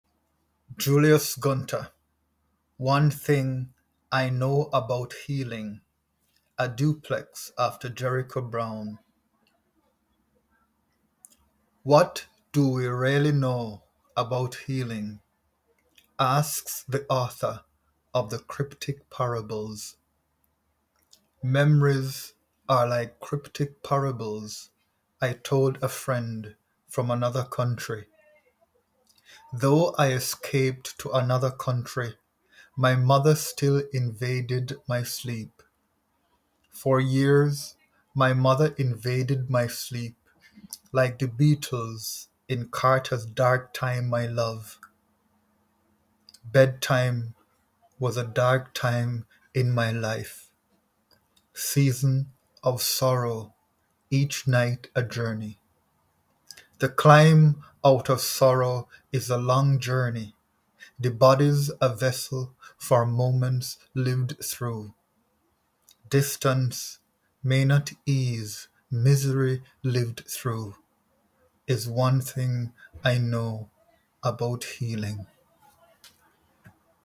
One Thing I Know About Healing (Duplex) by Juleus Ghunta
Poet’s Biography
Juleus Ghunta is a Jamaican poet, Chevening Scholar and children’s writer. His poems have appeared in The Missing Slate, Moko, Wasafiri, Anomaly, Chiron Review, Pittsburgh Poetry Review, and other journals. Ghunta won the Catherine James Poetry Prize in 2017 and was shortlisted for the Wasafiri New Writing Prize in 2022 and the Small Axe Poetry Prize in 2015 and 2016. He is the co-editor of two issues of Interviewing the Caribbean ( UWI Press).

This poem is a Duplex, a form created by American poet Jericho Brown. A Duplex has 14 lines, each line has 9 to 11 syllables. The first line must be echoed in the last, the second echoed in the third and so on. In this Duplex, I reflect on sleeplessness as an effect of (maternal) abuse, looking at my experience and how physically escaping does not always lead to psychological relief.

Poem Text
What do we really know about healing? –
asks the author of The Cryptic Parables.

Memories are like cryptic parables
I told a friend from another country.

Though I escaped to another country
my mother still invaded my sleep.

For years my mother invaded my sleep like
the beetles in Carter’s Dark Time My Love.

Bedtime was a dark time in my life,
season of sorrow, each night a journey.

The climb out of sorrow is a long journey;
the body’s a vessel for moments lived through.

Distance may not ease misery lived through
is one thing I know about healing.

‘Season of sorrow’ comes from This is the Dark Time My Love by Martin Carter.
‘Moments lived through’ comes from Citizen: An American Lyric by Claudia Rankine.